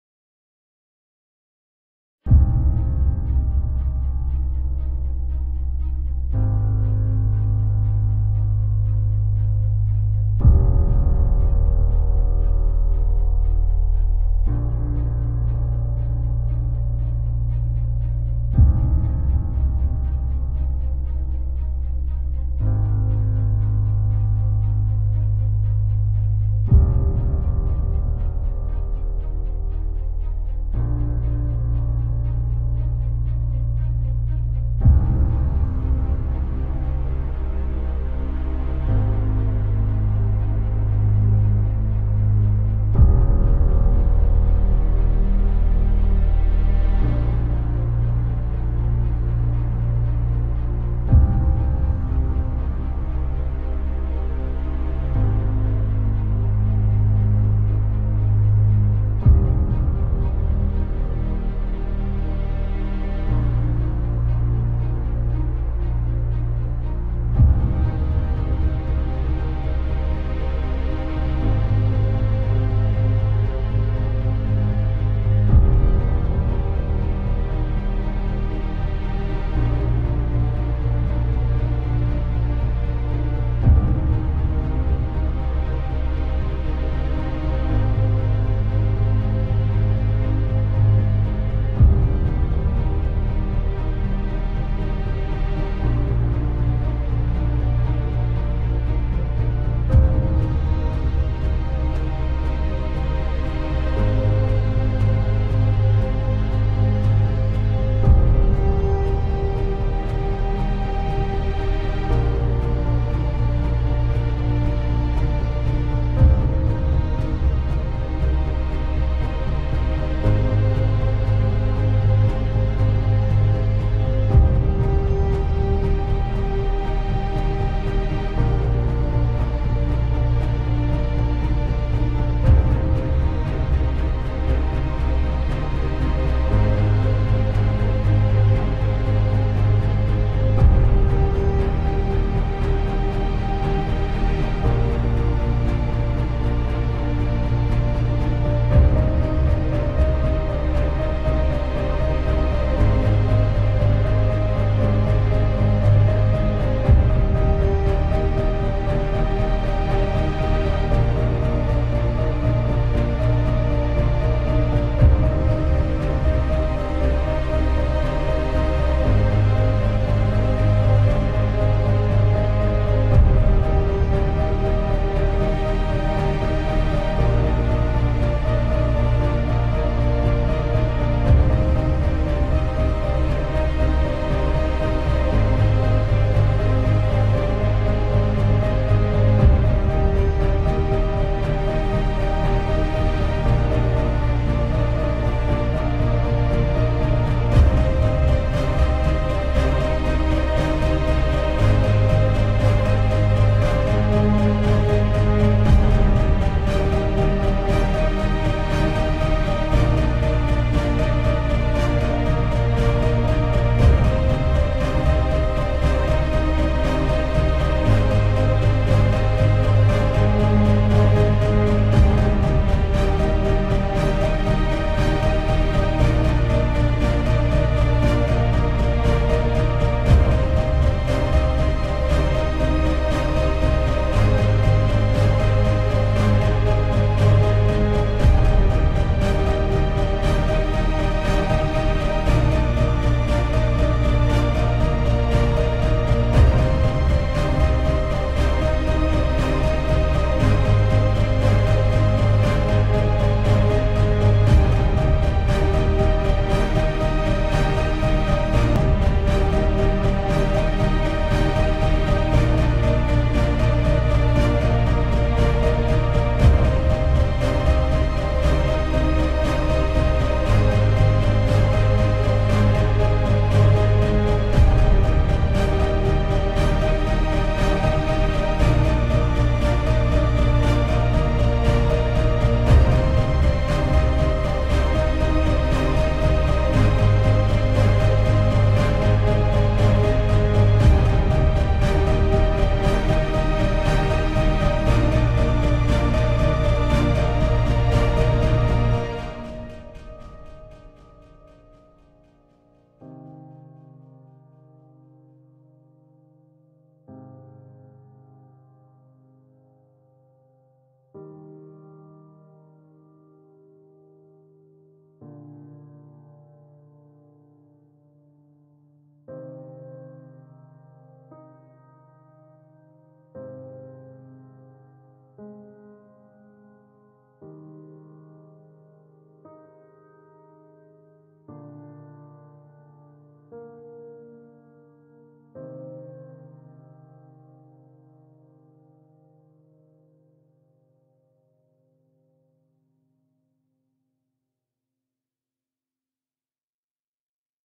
Playmobil custom Pausanias Unbound
An artistic homage to the great Spartan general sanctified and unbound from his accusators through his martyrdom.
The custom (non Playmobil) helmet and shield sculpts are made by Knights Workshop
The custom kopis (sword) sculpt is made by Playmalone
I enjoy modifying Playmobil -it is my hobby- and I hope that you enjoy my customizations.
It will help me grow and expand what I do!